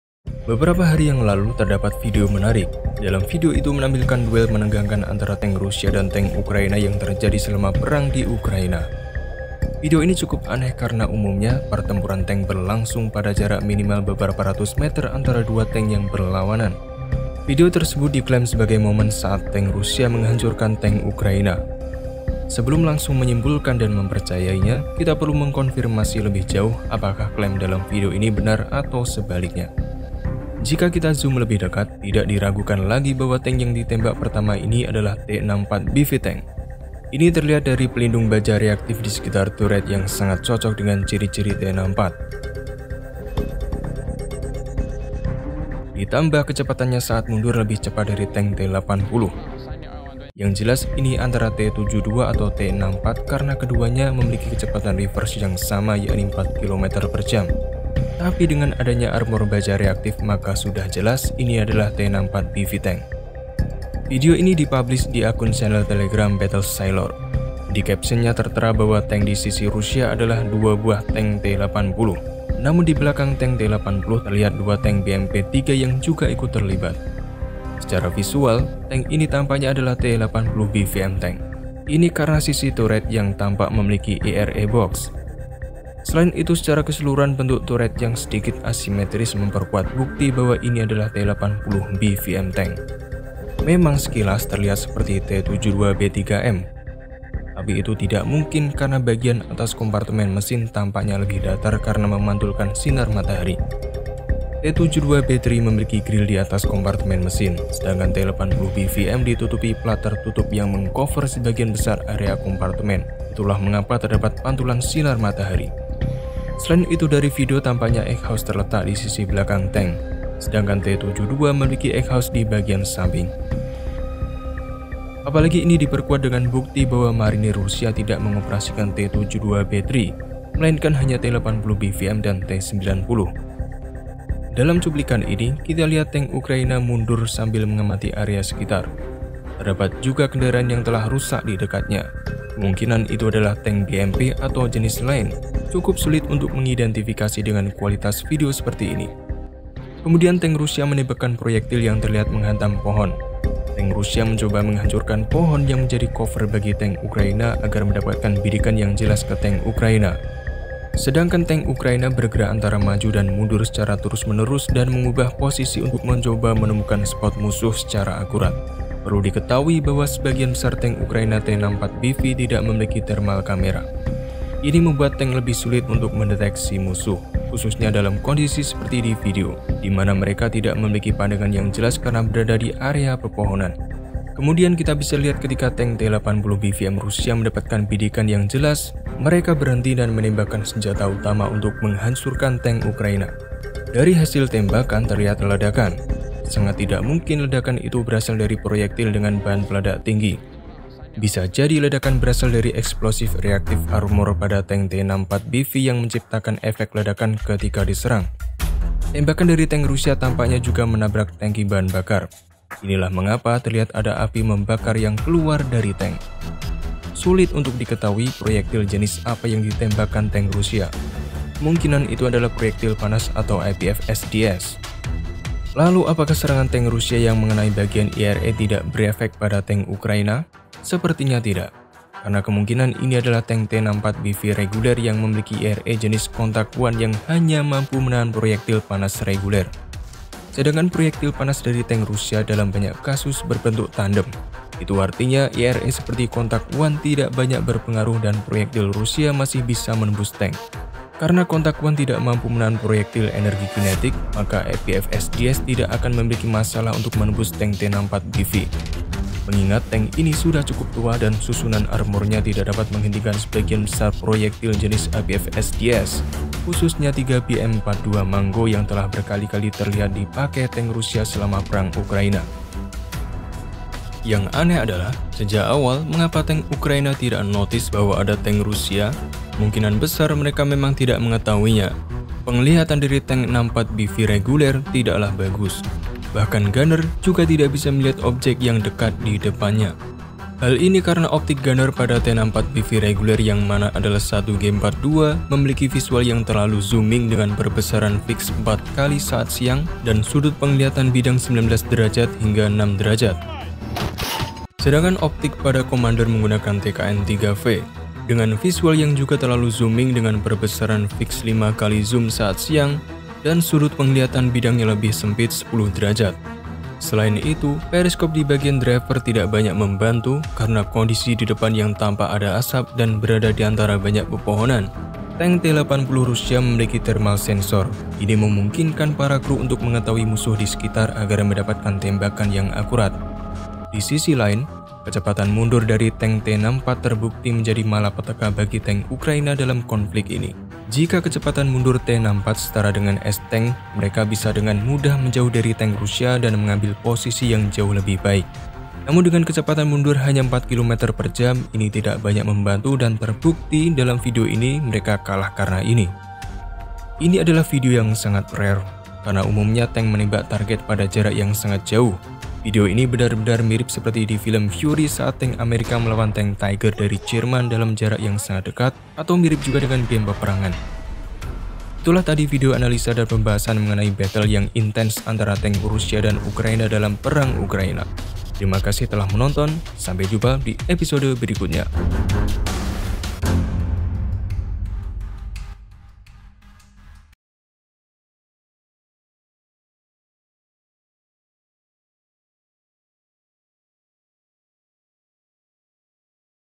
Pertempuran Dramatis Tank Russia vs Tank Ukraina | War Analysis
Pertempuran Dramatis Tank Russia vs Tank Ukraina.

Beberapa hari yang lalu terdapat video menarik, dalam video itu menampilkan duel menegangkan antara tank Rusia dan tank Ukraina yang terjadi selama perang di Ukraina. Video tersebut diklaim sebagai moment saat tank Rusia menghancurkan tank Ukraina, sebelum langsung menyimpulkan dan mempercayainya, kita perlu mengkonfirmasi lebih jauh, apakah klaim dalam video ini benar atau sebalilknya.